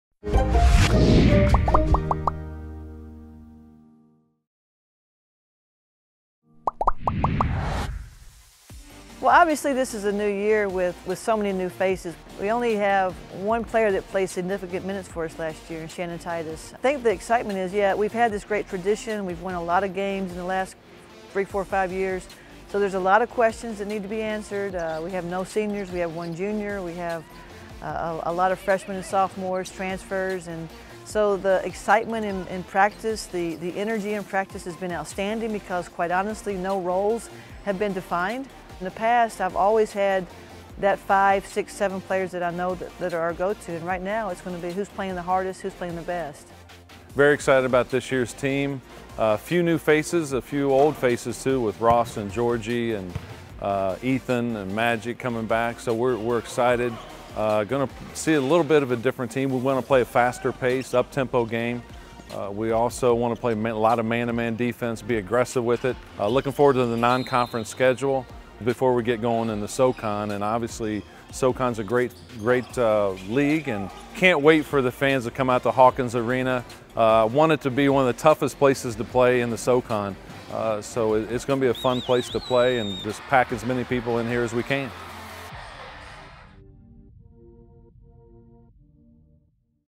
Mercer Basketball 2019-2020 Season Preview | This Week @ Mercer
The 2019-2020 men’s and women’s basketball seasons get under way this week. Women’s head coach Susie Gardner and men’s coach Greg Gary are pumped to tip off the season and anxious for Mercer fans to head to Hawkins Arena for the exciting action.